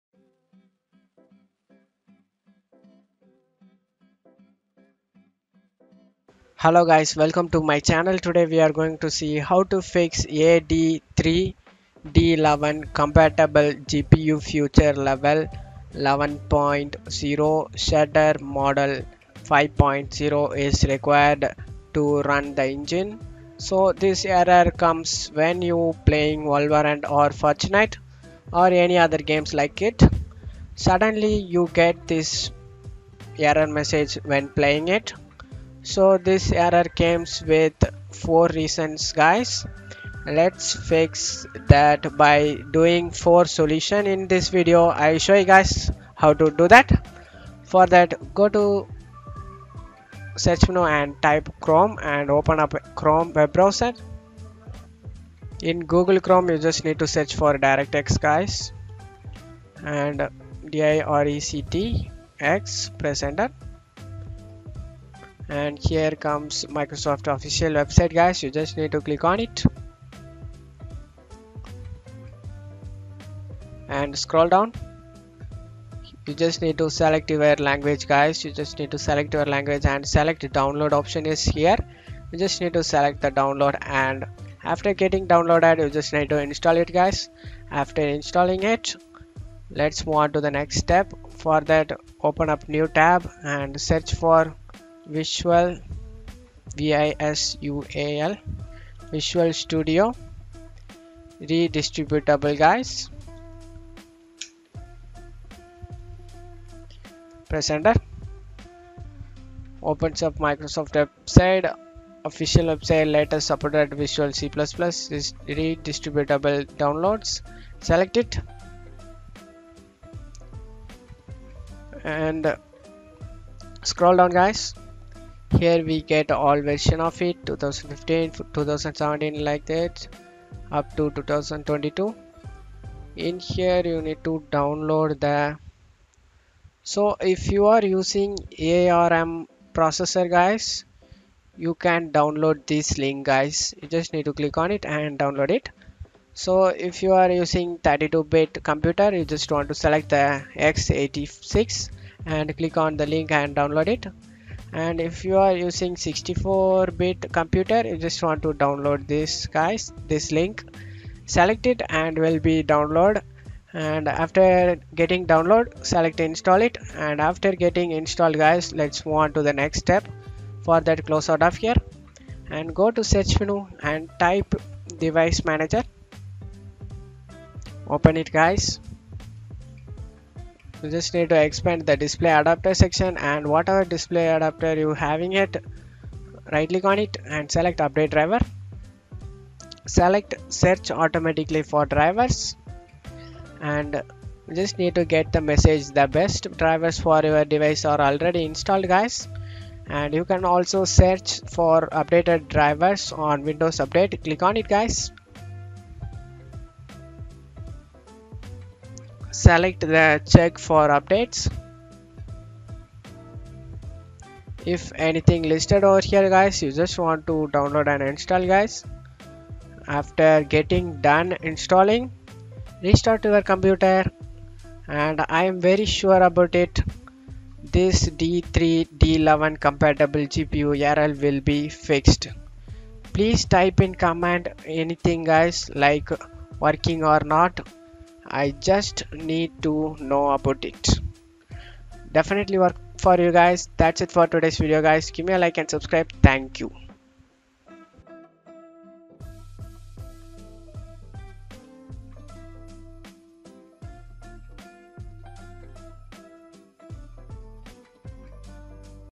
Fixing "A D3D11 Compatible GPU is Required to Run the Engine" Error (100% Working Solution)
Fixing "A D3D11 Compatible GPU (Feature Level 11.0 Shader Model 5.0) is Required to Run the Engine" Error

*Make Sure U Watched The Whole Video*

In This Video :
A D3D11 Compatible GPU (Feature Level 11.0 Shader Model 5.0) is Required to Run the Engine